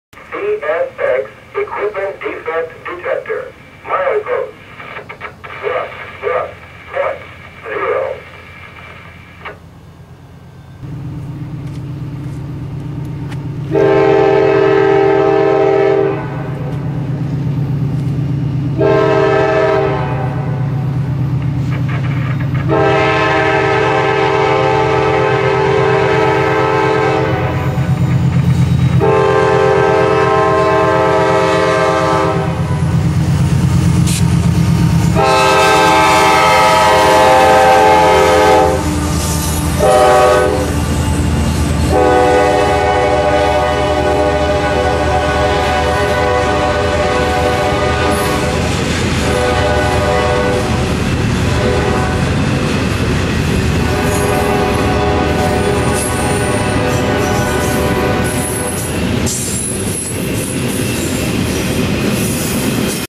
CSX Q142 at Folkston
CSX Q142 at Folkston,GA in Oct, 07 with a CSX AC6000CW and a CSX SD40-2